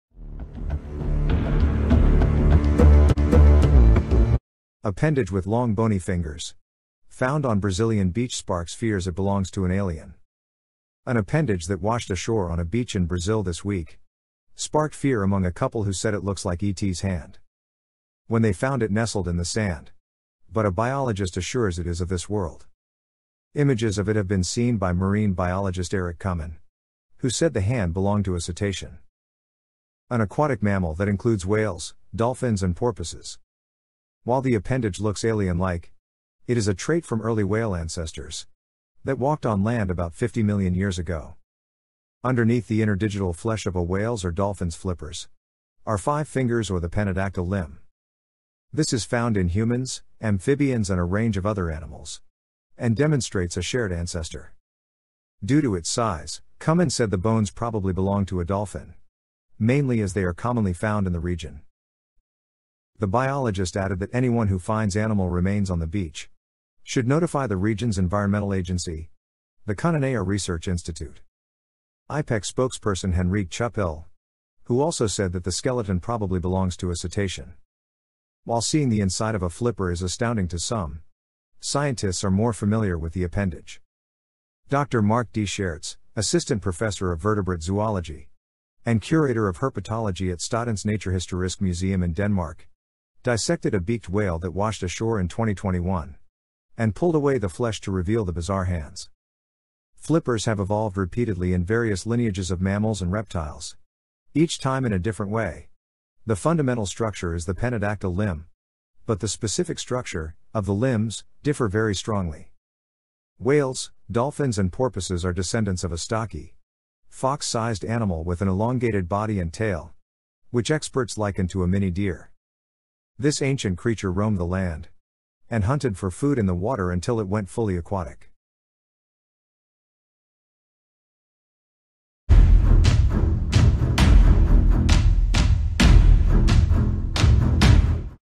Appendage that Looks Alien Hand Found on Brazilian Beach
An appendage that washed ashore on a beach in Brazil this week sparked fear among a couple who said it 'looks like ET's hand' when they found it nestled in the sand - but a biologist assures it is of this world.
Images of it have been seen by marine biologist Eric Comin who said the hand belonged to a cetacean - an aquatic mammal that includes whales, dolphins and porpoises.
While the appendage looks alien-like, it is a trait from early whale ancestors that walked on land about 50 million years ago.
Underneath the interdigital flesh of a whale's or dolphin's flippers are five 'fingers' or the pentadactyl limb.
This is found in humans, amphibians and a range of other animals and demonstrates a shared ancestor.

IMAGE CREDIT/COURTESY:
JAM PRESS/LETICIA GOMES SANTIAGO